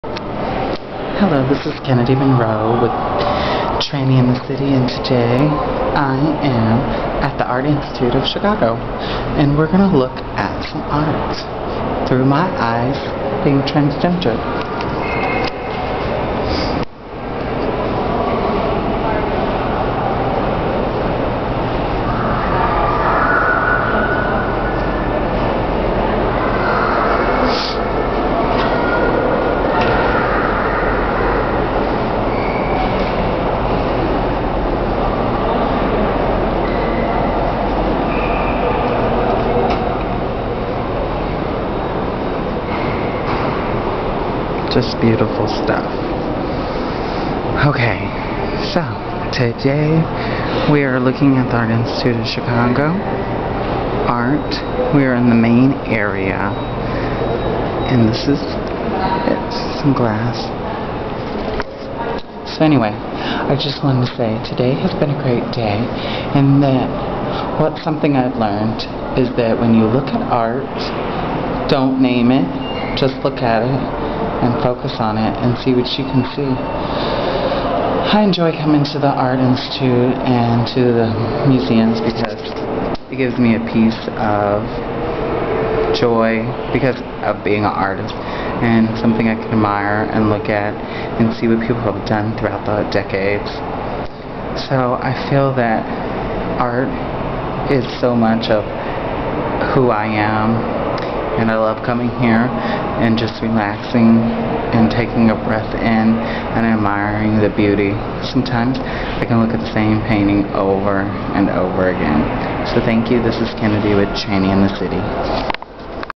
Tranny in the City: Episode 6 part 1
Kennidi ends her work day by going into the Art Institute of Chicago to relax.